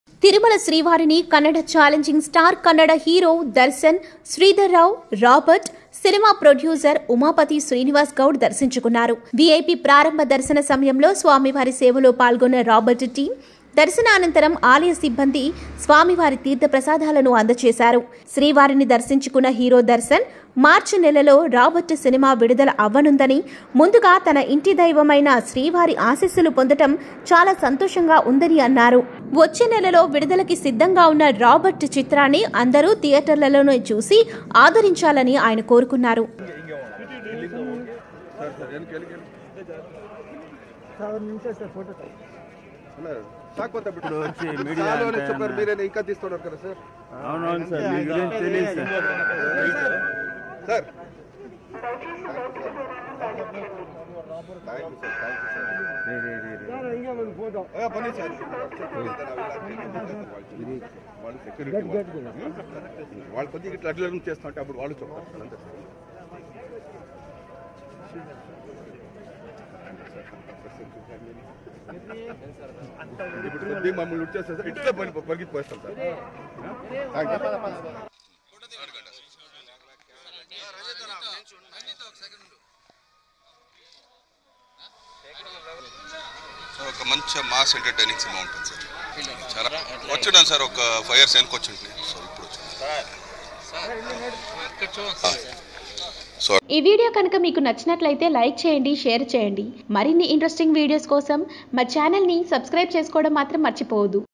Kannada Hero Darshan Visits Tirumala Tirupati Temple | Hero Darshan About Roberrt Movie
Watch, Kannada Hero Darshan Visits Tirumala Tirupati Temple | Hero Darshan About Roberrt Movie